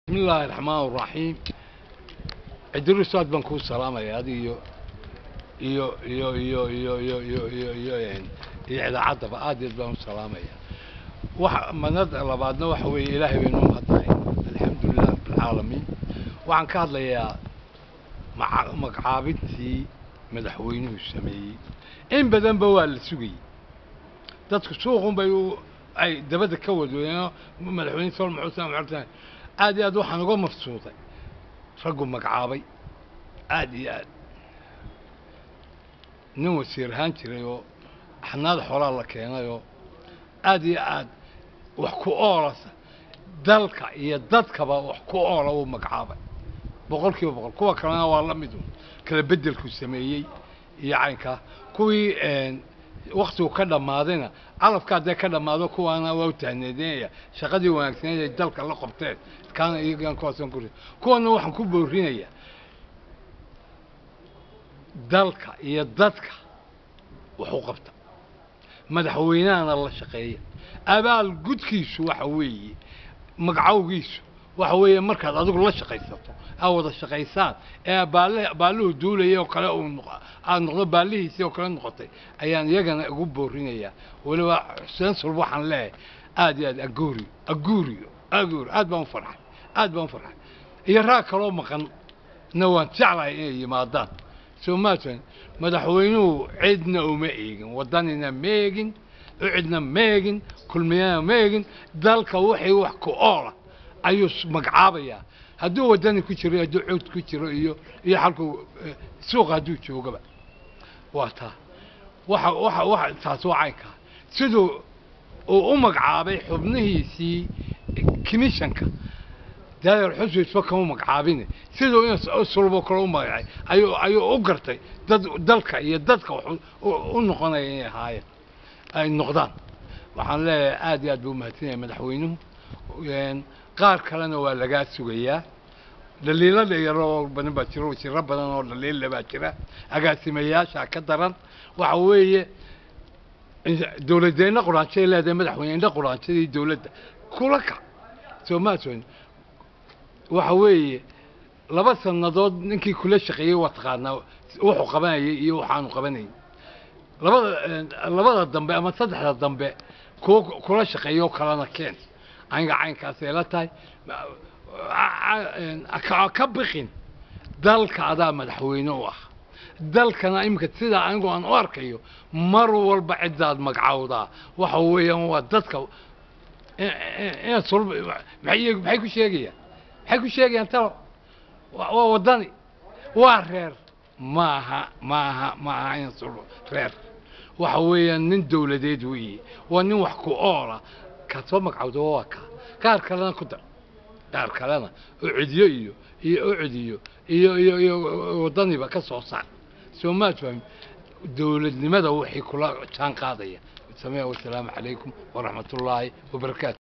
XIL C/RAXMAAN SHAADH AYAA SICIID SULUB MAGACAABISTIISA BOGAADIYEY.
XIL C/RAXMAAN SHAADH AYAA SICIID SULUB MAGACAABISTIISA BOGAADIYEY,Madaxweynaha QARANKANA UGA MAHADCELIYEY ISKU SHAANDHEYNTII UU KU SAMEEYEY GOLIHIISA WASIIRADA..